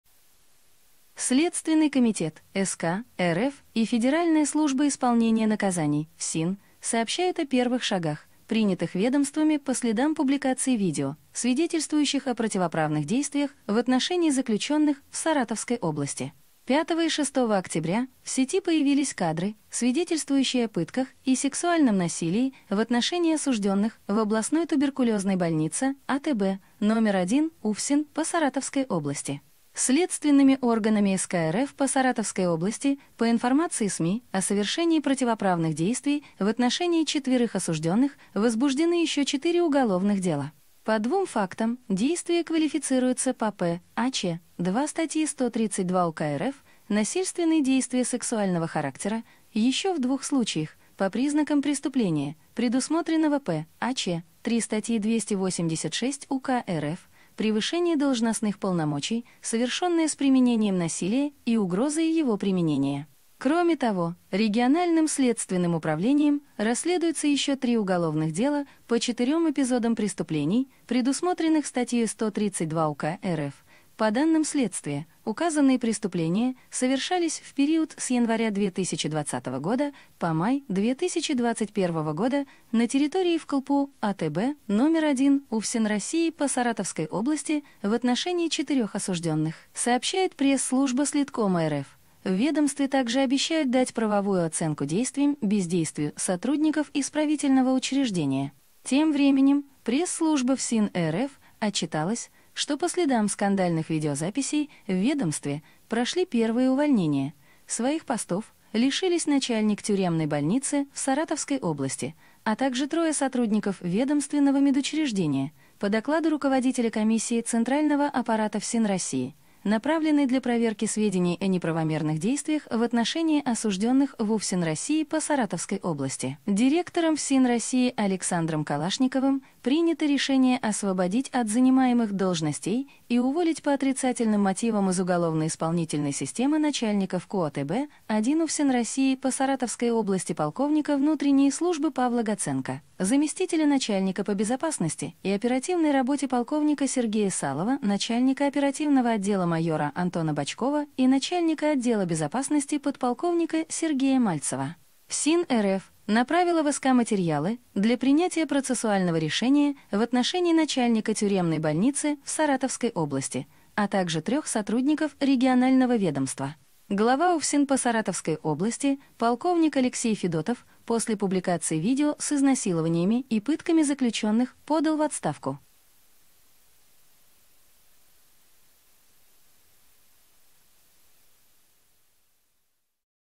Громкое дело о пытках заключенных в Саратовской области: Следком и ФСИН сообщают о первых шагах
– новости политики.

2 дня назад - В среду, в вечернем эфире «Беларусь 1» появилось новое ток-шоу «Понятная политика плюс»  - 60 минут прямой эфир: